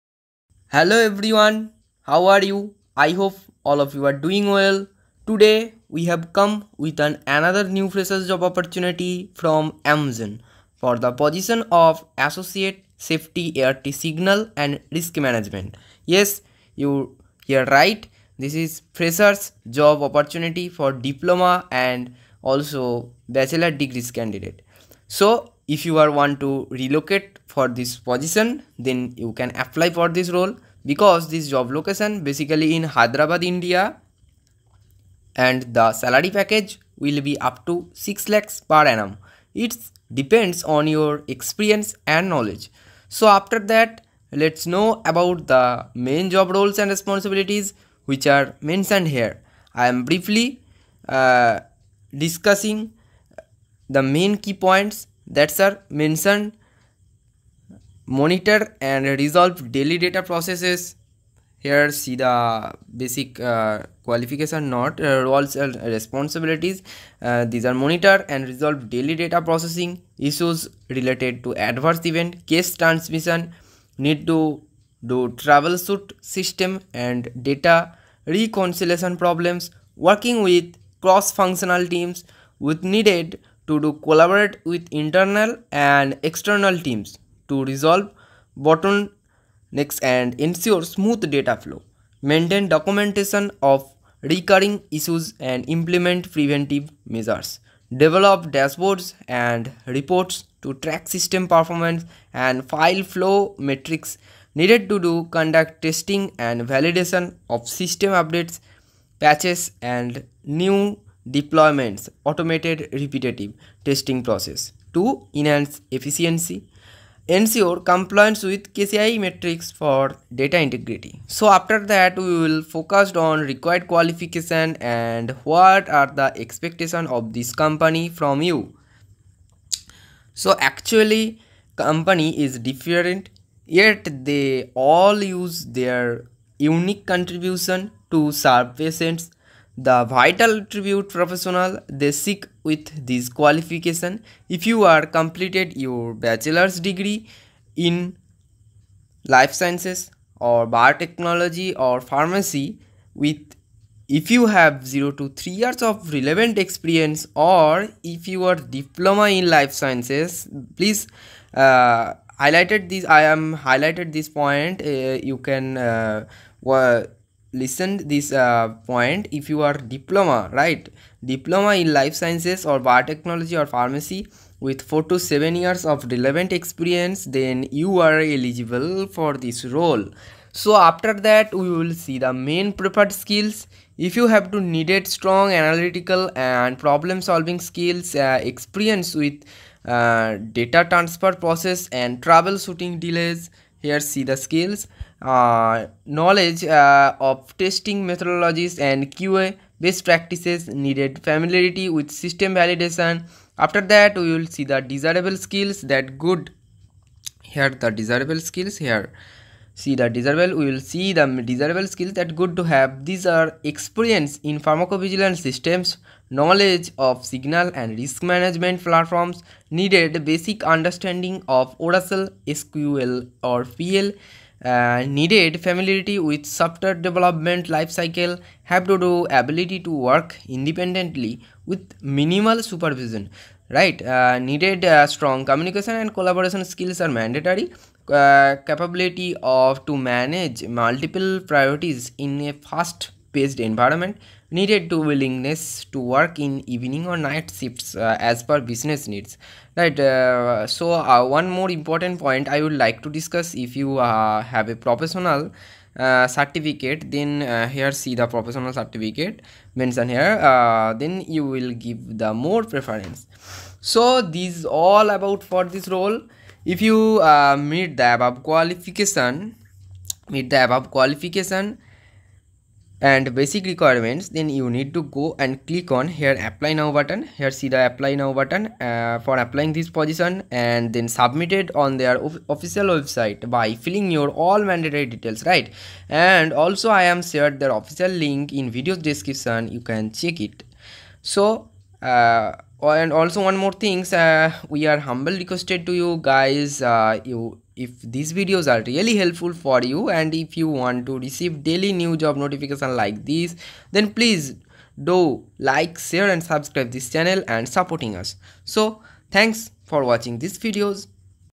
Fresher & Experience Pharmacovigilance | Diploma, Bachelor degree in pharmacy | Amgen Hiring | 2025
Job name : Associate, Safety ART - Signal & Risk Management

Organization : Amgen

Qualification : Bachelor’s degree and 0 to 3 years of Life Science/Biotechnology/Pharmacology/Information Systems experience
Or
Diploma and 4 to 7 years of Life Science/Biotechnology/Pharmacology/Information Systems experience

Skills : Strong analytical and problem-solving skills to diagnose and resolve system-related issues.
Experience with data transfer processes and tackle stuck or delayed data files.
Knowledge of testing methodologies and quality assurance standard processes.
Proficiency in working with data analysis and QA tools.
Knowledge of Validation of systems

Location : Hyderabad, India

For Application please type interested then i will share the Application link